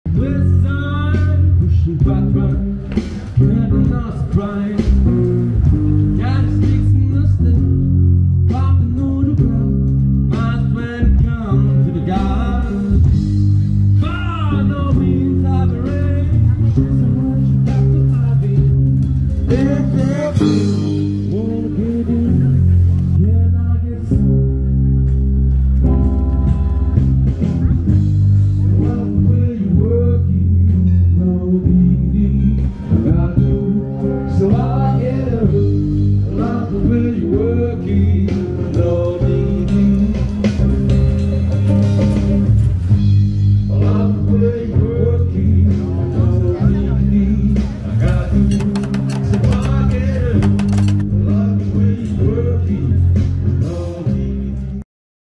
Marché des saveurs
Dernier marché des saveurs samedi sur la place de la Basilique